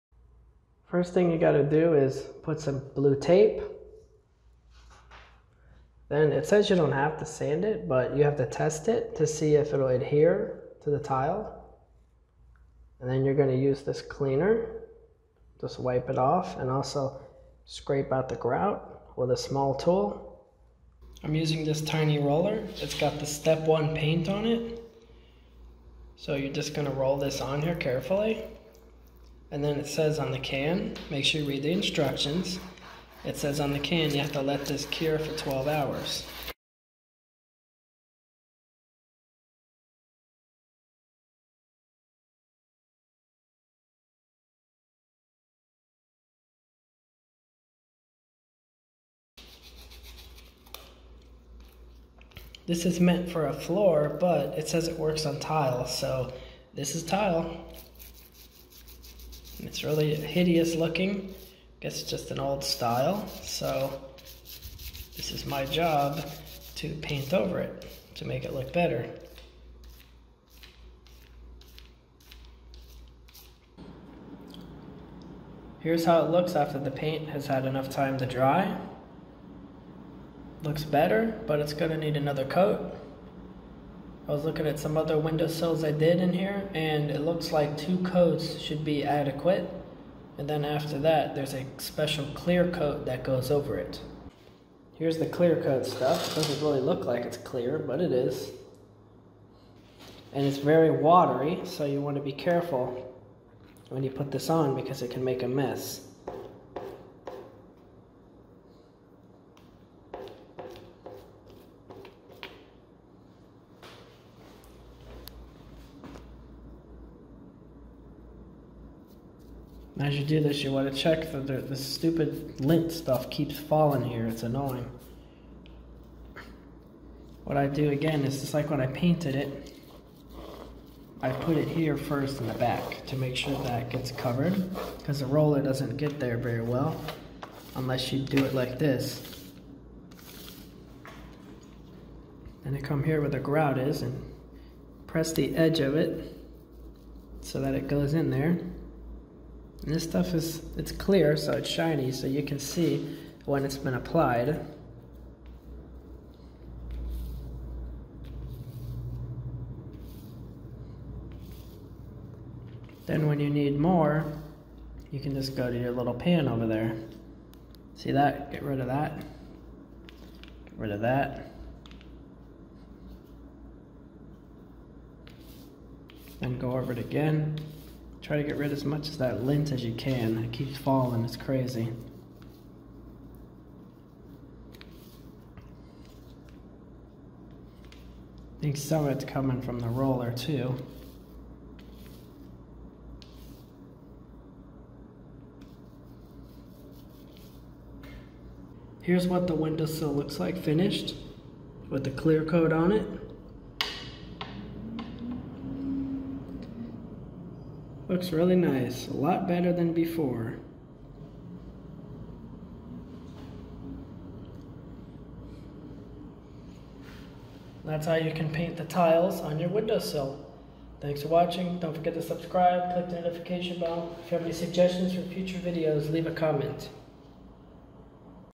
How to paint tile window sills
Learn how to paint these old ugly looking tiles with tile paint.